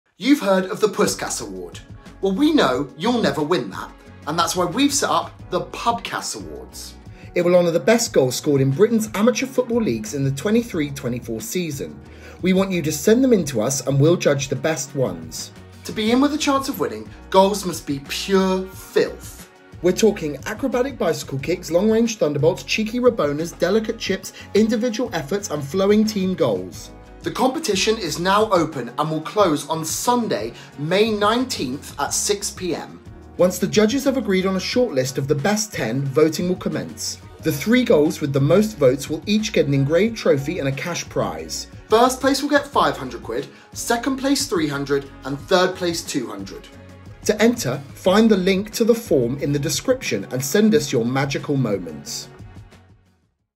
🏆 Pubkas Awards: Send us your best goals from amateur leagues – you could win a CASH prize
You've heard of the Puskás Award – now it's time for the Pubkas Awards.
Just like FIFA's annual trophy, the brand new accolade recognises the beautiful game's "most beautiful" goals.

But solo efforts and screamers from GOAT contenders such as Kylian Mbappe, Lionel Messi and Seamus Coleman won't be considered.

Instead, the Pubkas Awards will honour the finest goals scored in amateur football leagues only.

So if you've captured a Sunday morning worldie or a pub team cracker on camera in the 2023/24 season, we want to see it.

To be in with a chance of winning, goals must be pure filth – in a good way. We're talking acrobatic bicycle kicks, long range thunderbolts, cheeky rabonas, delicate chips, individual efforts and flowing team goals.

Flukes, deflected strikes and any goals involving the dark arts or foul play will be immediately booted out of contention by our panel of expert judges tasked with shortlisting the best of the bunch.

The Puskás Award, named after legendary 1950s Real Madrid and Hungary forward Ferenc Puskás, is handed out "without distinction of gender" – and the Pubkas Awards are similarly inclusive in that respect.

Goals scored in both the men's and women's game, junior, open age and veterans leagues, will be accepted. Crucially, they have to have been scored in amateur competitions.

The competition is now open and will close at 6pm on Sunday May 19, which should be around the time the last ball is kicked in the Premier League this season. Once the judges have agreed on a shortlist of the best 10, voting will commence.

The three goals with the most votes at the end of the process, which closes at at 6pm on Sunday June 16, will each get an engraved trophy and a cash prize – as well as the glory, of course. First place will get £500, second place £300 and third place £200.

Send in your entries now to be in with a chance of winning. After all, if you don't shoot...